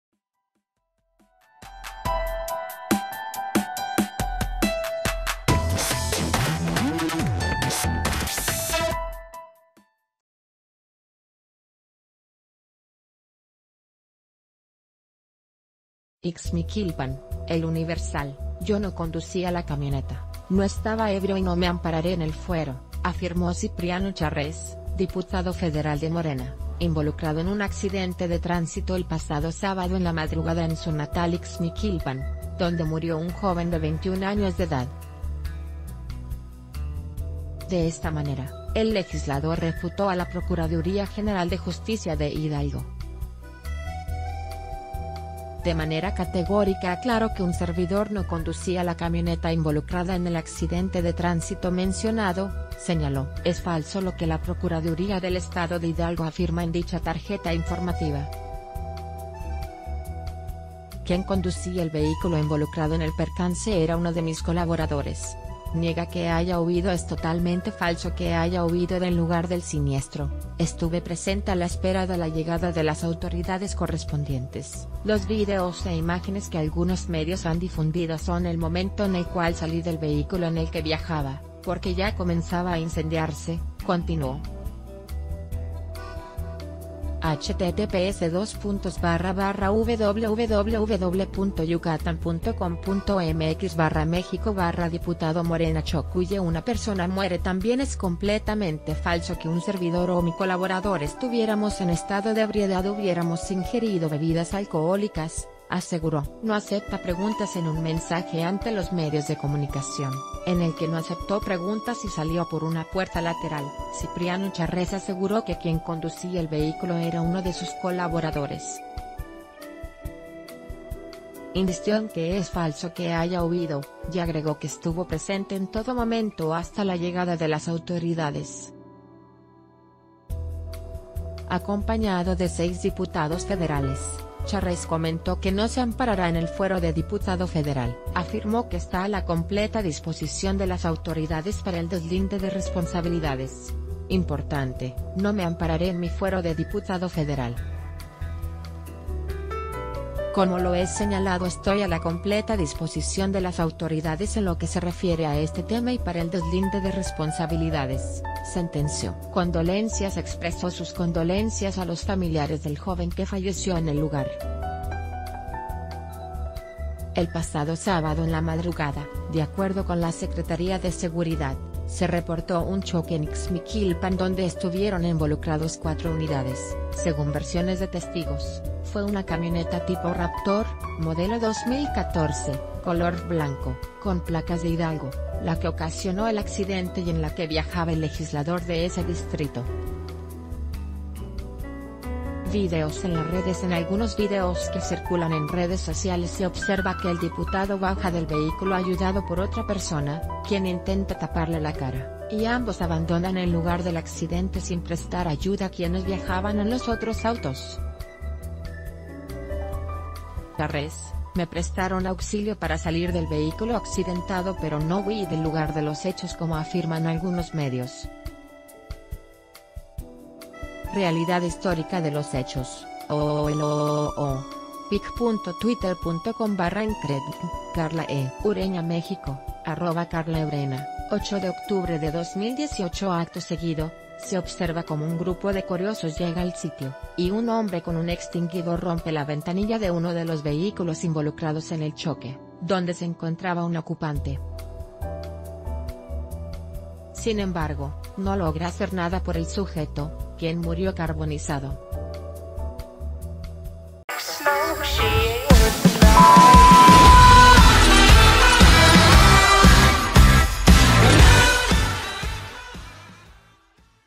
No me ampararé en el fuero: diputado involucrado en trágico accidente - El Diario de Yucatán
IXMIQUILPAN (El Universal).— Yo no conducía la camioneta, no estaba ebrio y no me ampararé en el fuero, afirmó Cipriano Charrez, diputado federal de Morena, involucrado en un accidente de tránsito el pasado sábado en la madrugada en su natal Ixmiquilpan, donde murió un joven de 21 años de edad. De e...